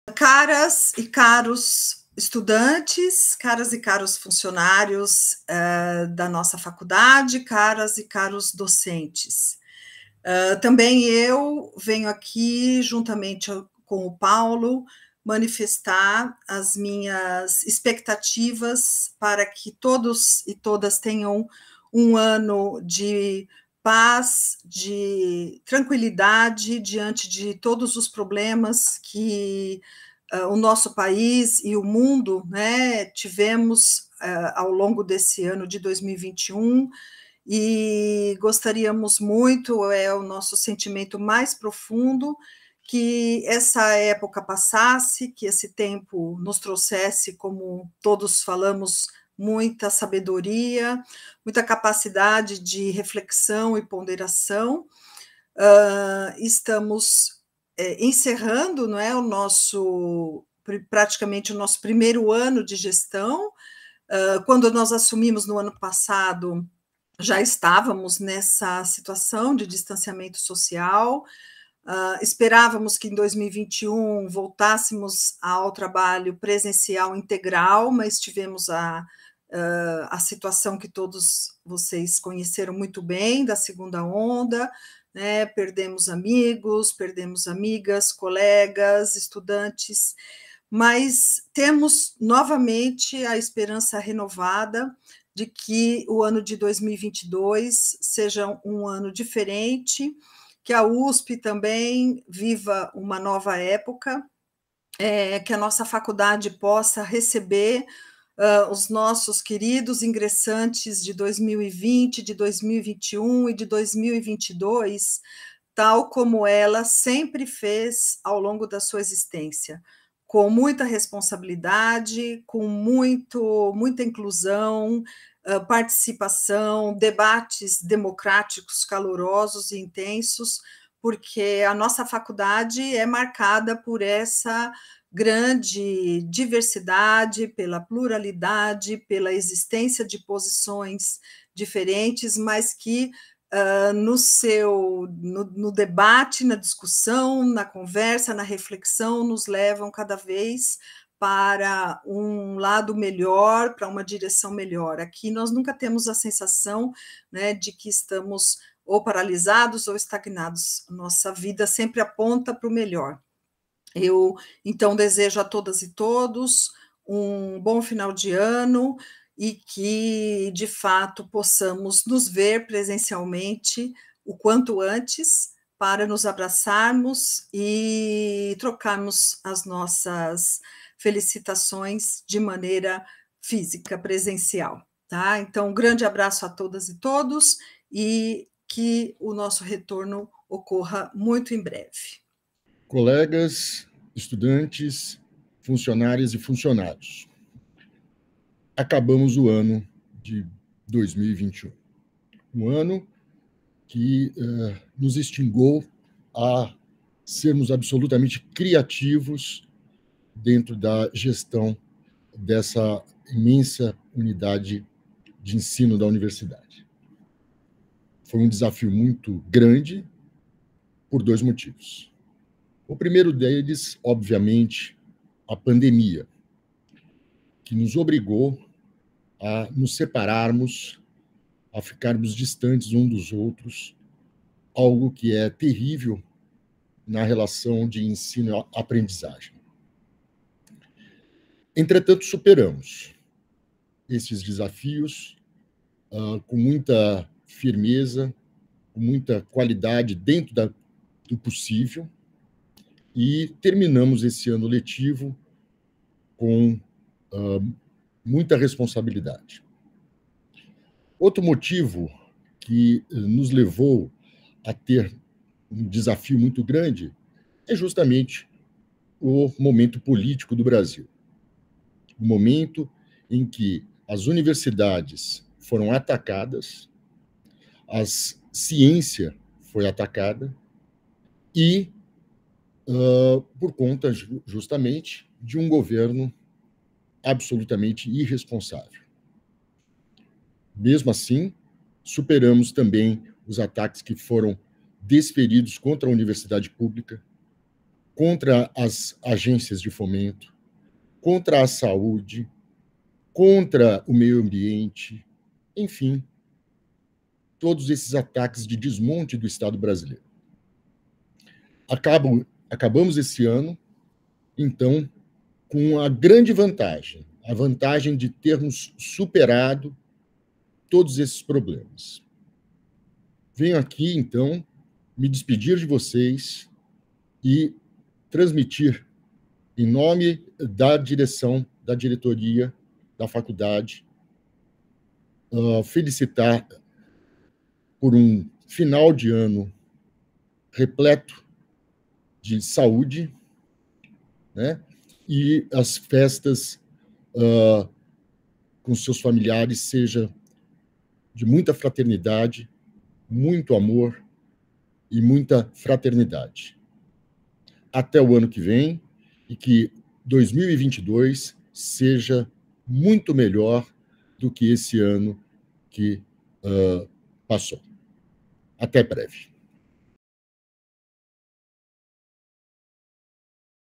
Mensagem de fim de ano da Direção da Faculdade
A vice-diretora Ana Paula Torres Megiani e o diretor Paulo Martins desejam um excelente fim de ano e que 2022 seja um ano melhor para todos.